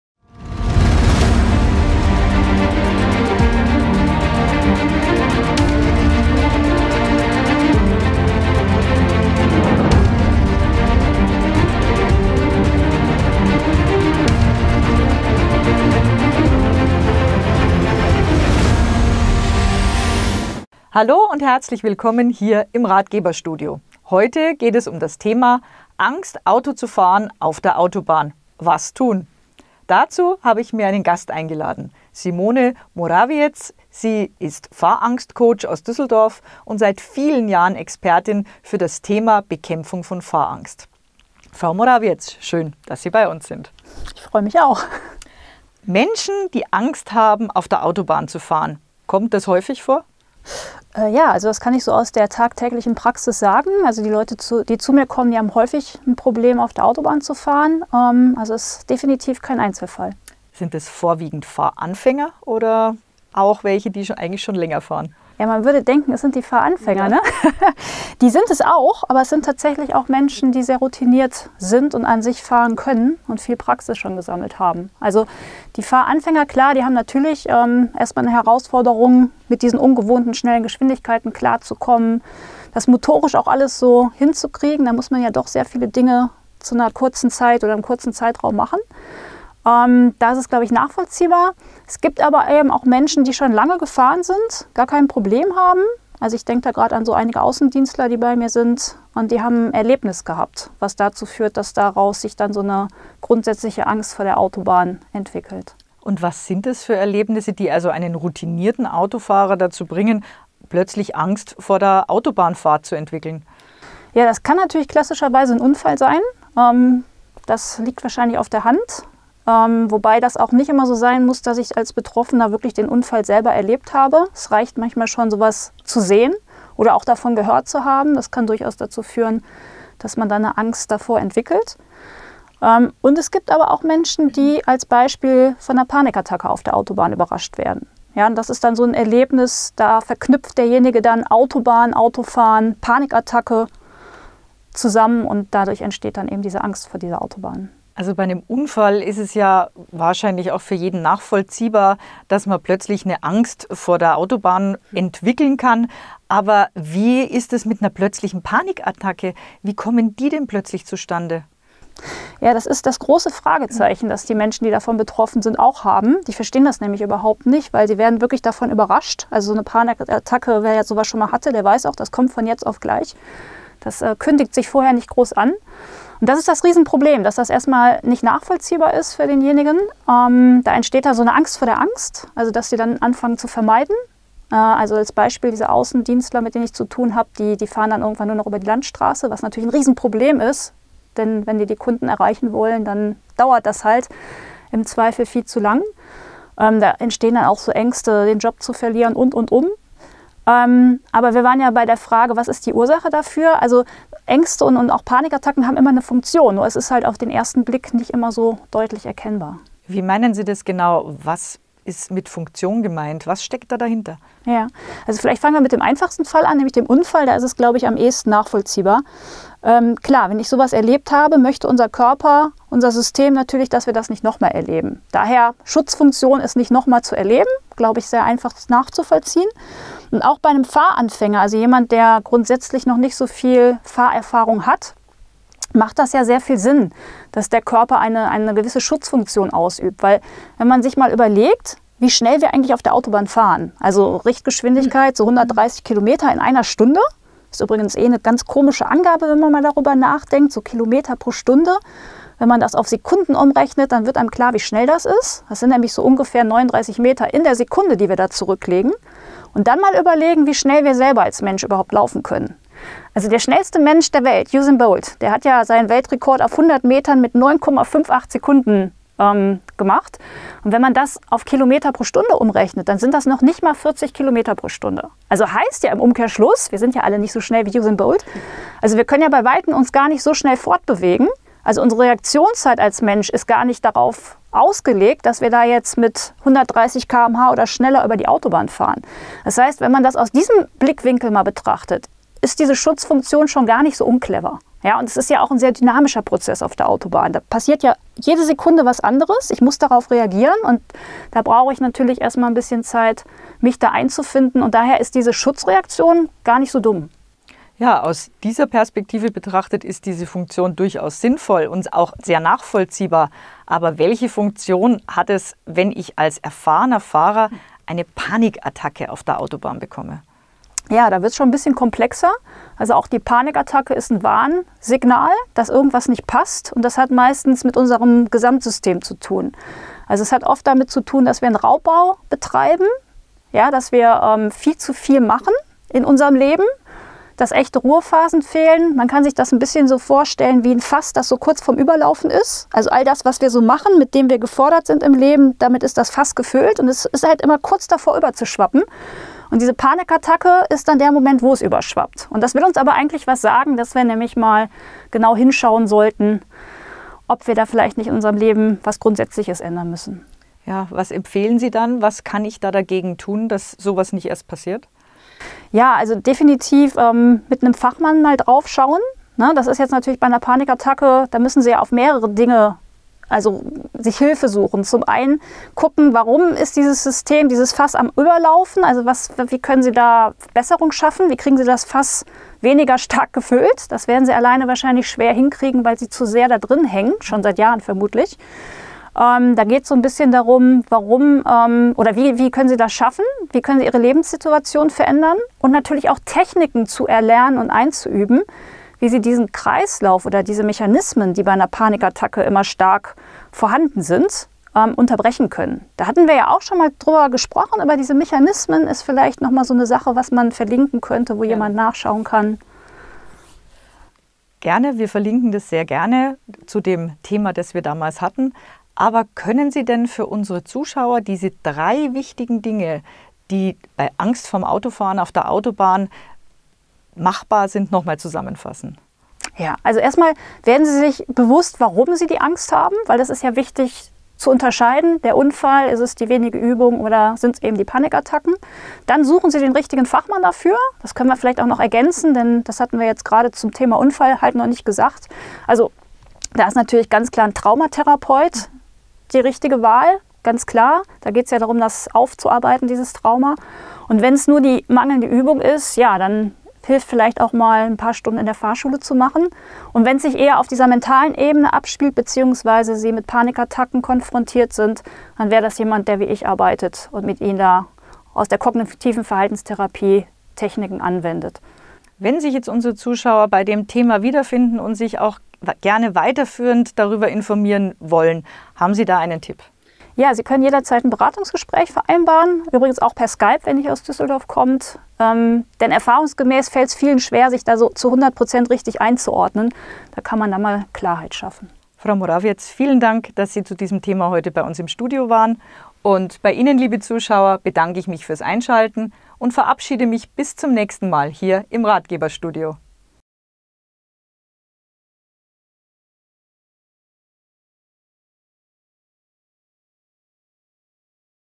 Angst Auto zur fahren auf der Autobahn - Zu Gast im Ratgeberstudio die Expertin Simone Morawietz
Angst Auto zur fahren auf der Autobahn  - was tun? Als Fahrangstcoach gibt Simone Morawietz Auskunft im Interview im Ratgeberstudio.
Wer ist das Klientel, das Angst vorm Autofahren hat? Wie komme ich raus aus der Angst beim Autofahren auf der Autobahn? Angst als Schutzreaktion - eine andere Perspektive. Was mache ich, wenn ich beim Autofahren eine Panikattacke auf der Autobahn habe? Welche Mechanismen lösen die Panikattacken aus?

Antworten auf diese Fragen und mehr zu diesem Thema finden Sie